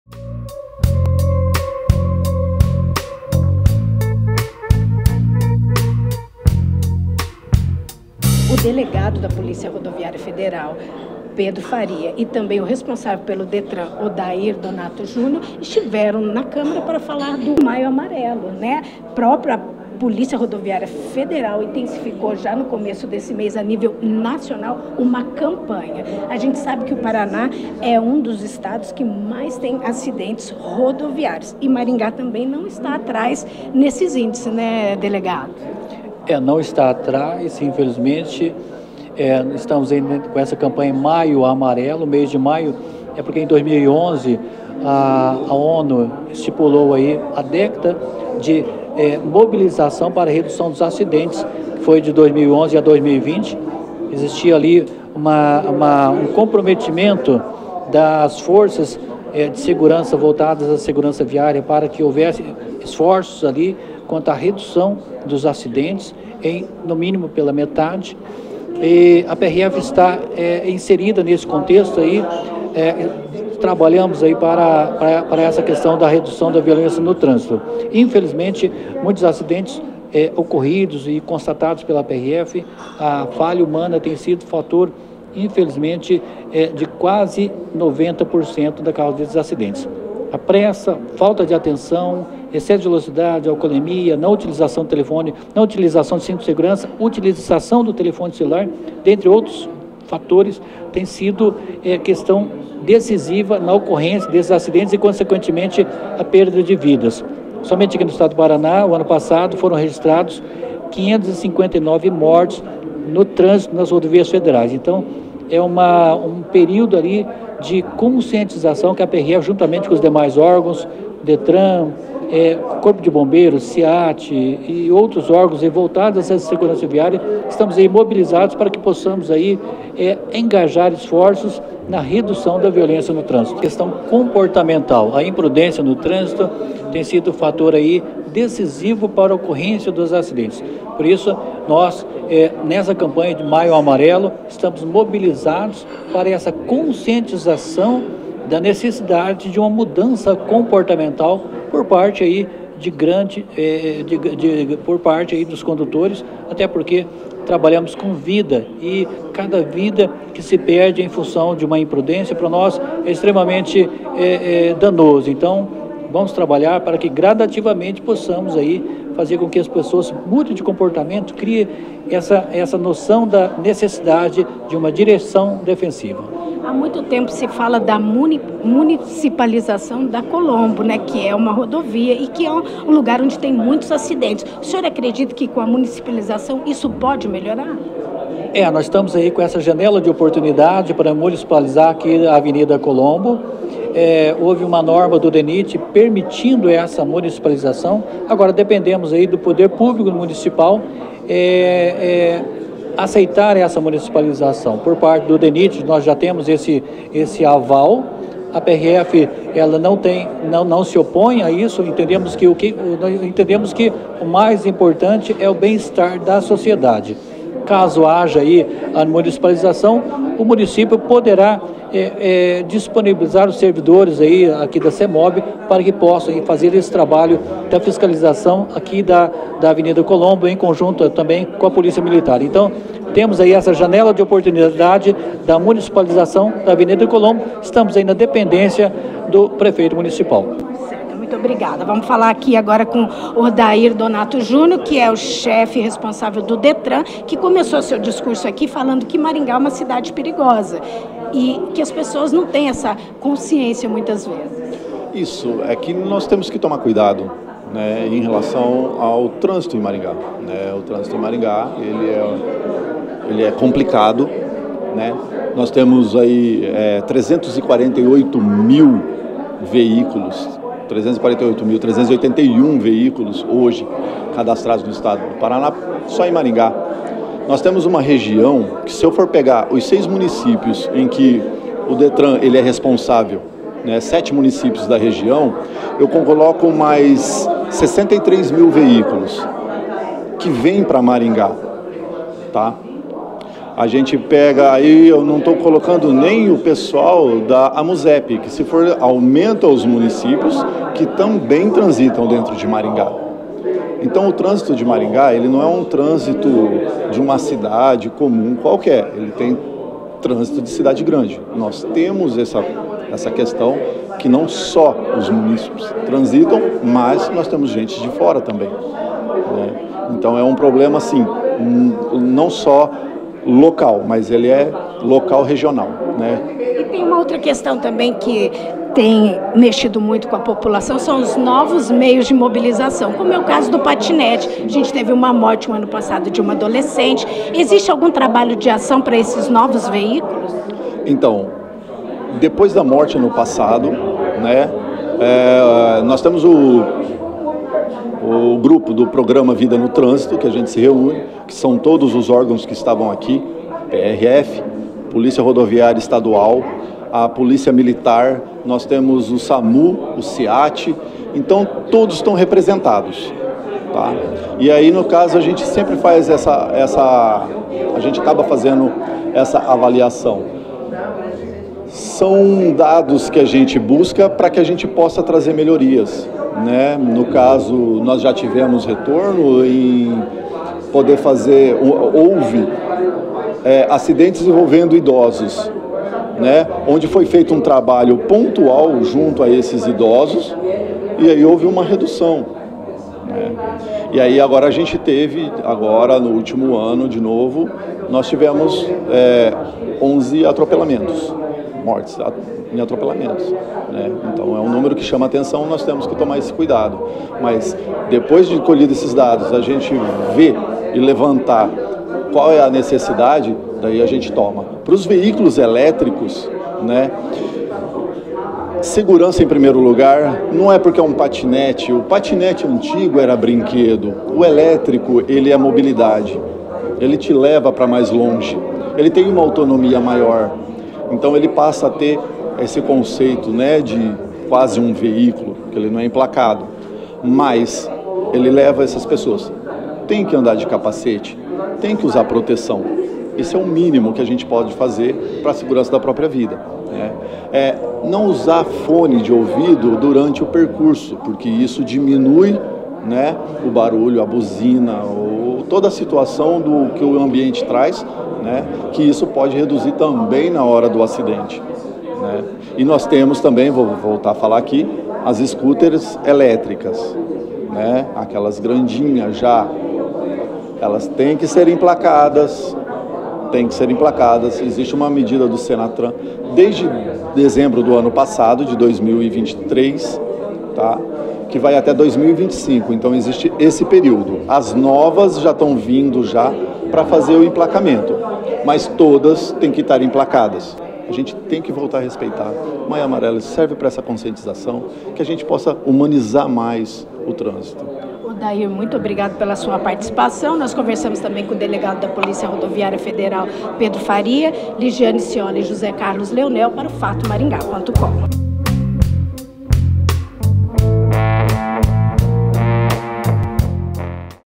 MAIO AMARELO: "559 paranaenses morreram em acidentes nas rodovias federais do Paraná só em 2023"
Dados foram apresentados na manhã desta quinta-feira, 2, pelo Inspetor Chefe da PRF/PR - Pedro Faria e pelo chefe da 13ª Ciretran Odair Donato Junior

O Chefe da Polícia Rodoviária Federal em Maringá - Inspetor Pedro Faria e o chefe da 13ª Ciretran - Odair Donato Junior, participaram da sessão ordinária da Câmara Municipal para apresentarem as ações do Maio Amarelo; campanha de conscientização que tenta reduzir o número de acidentes e vítimas no trânsito.

De acordo com as estatísticas, em todo o Brasil só ano passado 33 mil pessoas morreram vítimas de acidentes de trânsito. O Paraná é o terceiro colocado em número de acidentes no trânsito, e Maringá está entre as cidades que tem o trânsito mais violento do estado.

Pedro Faria falou da necessidade de respeitar as regras do trânsito. O Inspetor Chefe da PRF/PR em Maringá diz que 90% dos acidentes acontecem por imprudência dos motoristas. Faria explica que apesar das muitas campanhas de conscientização, a maioria dos motoristas ainda age como se a Lei devesse ser respeitada somente pelos outros e não por eles. Ele diz que comportamentos como falar ao celular enquanto dirige, excesso de velocidade e direção sob efeito de álcool são os maiores fatores de riscos de acidentes.

"A pressa, a falta de atenção, o excesso de velocidade, o abuso do álcool, uso de celular, o não uso de cinto de segurança, estão entre as principais razões das 559 mortes registradas nas rodovias federais do Paraná só no ano de 2023", afirma Pedro Faria.

O Inspetor Chefe da PRF/PR diz que uma das medidas que ajudaria muito a reduzir os acidentes no perímetro urbano de Maringá, seria a municipalização da fiscalização da BR 375 no trecho conhecido como avenida Colombo que cruza toda a cidade de Maringá.

Odair Donato Junior - Chefe da 13ª Ciretran de Maringá

O chefe da 13ª Ciretran de Maringá também falou sobre o trânsito na ótica urbana. Odair Donato Junior explica que Maringá tem uma frota enorme de veículos e que esse fluxo aumenta muito com a circulação de veículos que todos os dias circulam em Maringá vindos de cidades da região. "É um trânsito perigoso, mas não porque é mal concebido, mas porque as pessoas mesmo tendo as informações sobre como devem dirigir, continuam a agir com imprudência". Desde dezembro do ano passado

Uma das situações enfatizadas pelo chefe do trânsito de Maringá, é a novidade da circulação de patinetes e de motos elétricas. Odair diz que quem usa patinete precisa ter consciência, usar capacete e circular respeitando as leis do trânsito mesmo não sendo necessária a posse de habilitação. Sobre as motos e scooters elétricas, o chefe da Ciretran explicou que quem faz uso, tem que ter habilitação e que todos os veículos devem ser emplacados até dezembro de 2025.